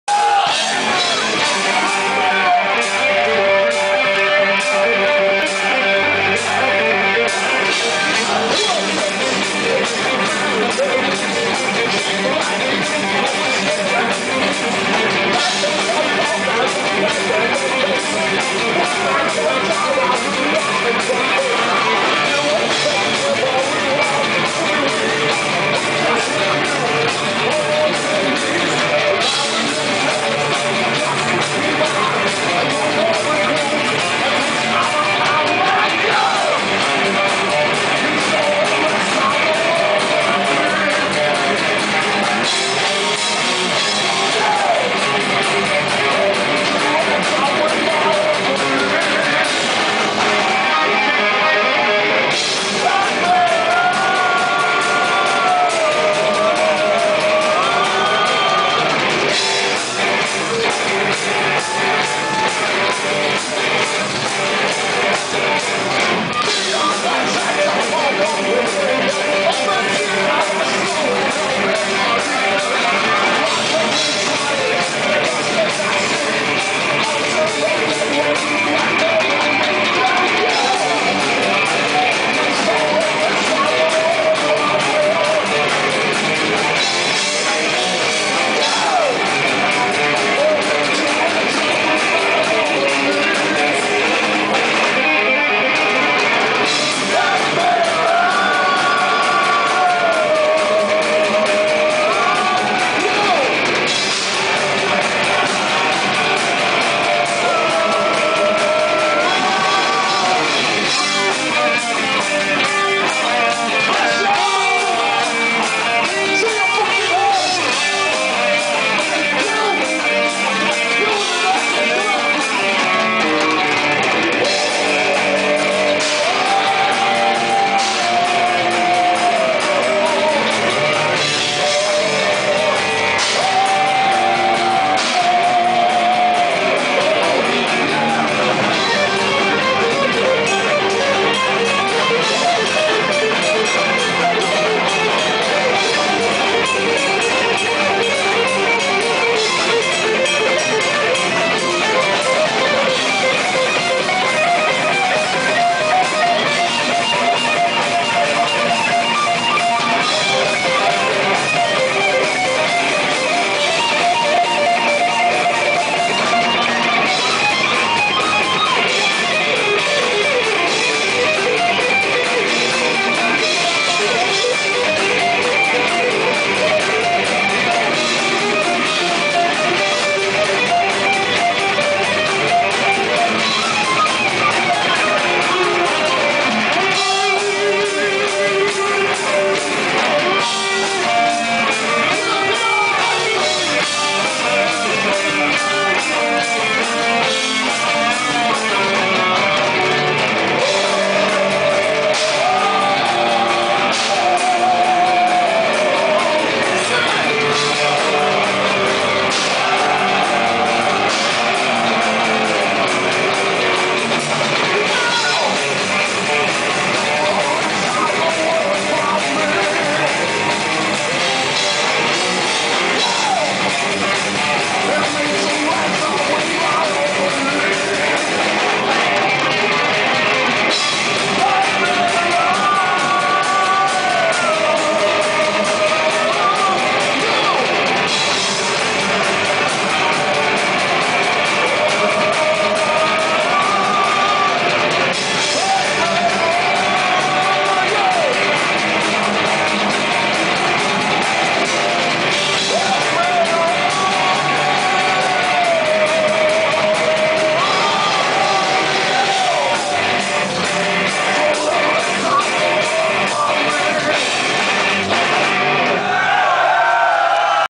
Blaze Bayley 2/4 - Live - Progresja / Warszawa 2009
Zapis fragmentu koncertu Blaze'a Bayleya z warszawskiej Progresji (2009)